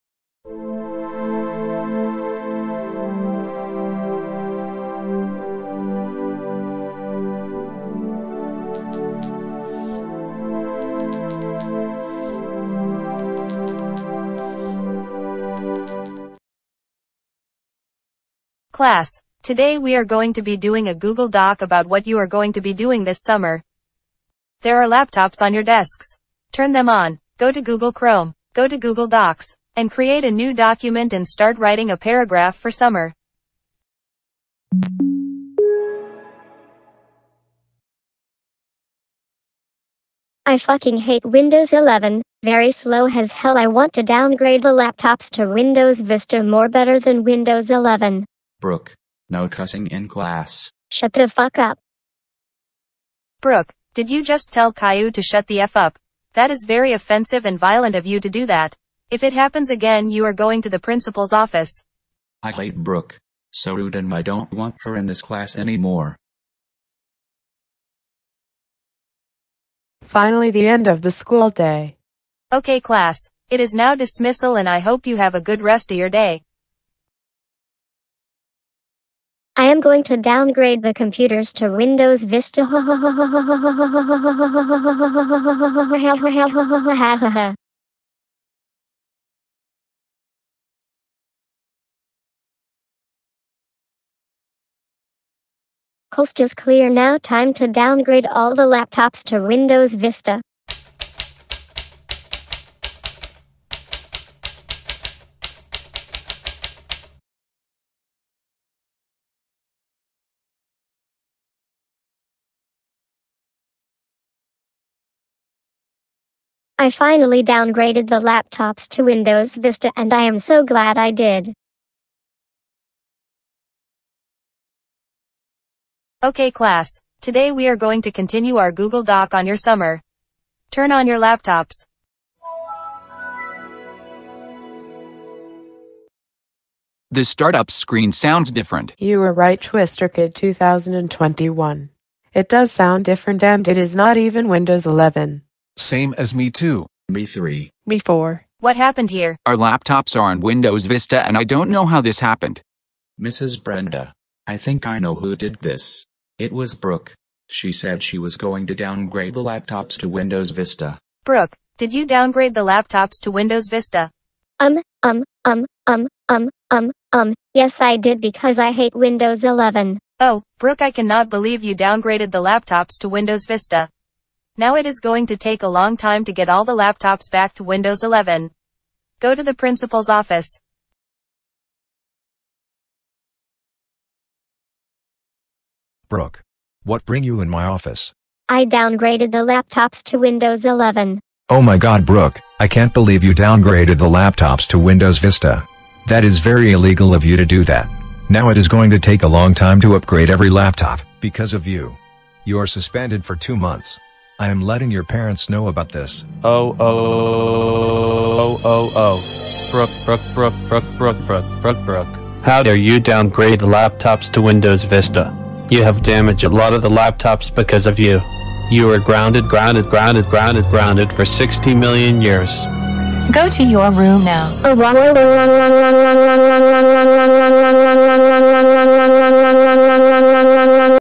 Brooke Downgrades The Laptop To Windows Vista and Gets Grounded
Brooke hates Windows 11 and she breaks in the school in the middle of the night to downgrade the laptops so everyone can lose their work